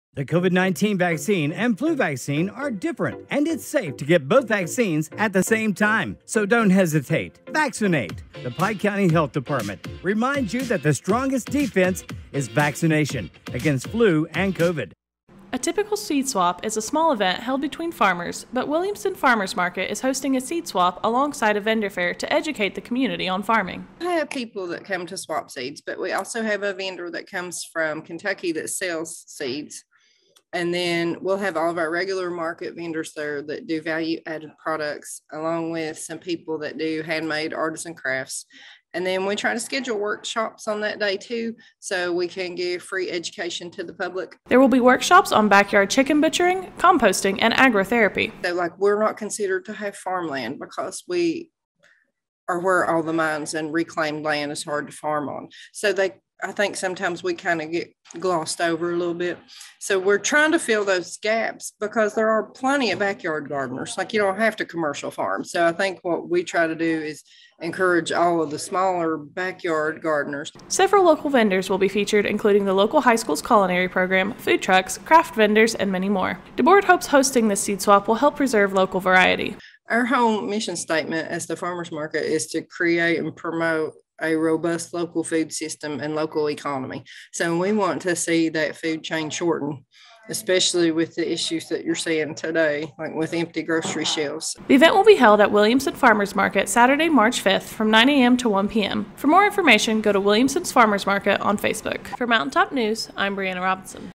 Williamson Farmer's Market to Host Seed Swap and Vendor Fair
The Williamson Farmer's Market hopes to educate the public on farming by hosting a vendor fair alongside a seed swap.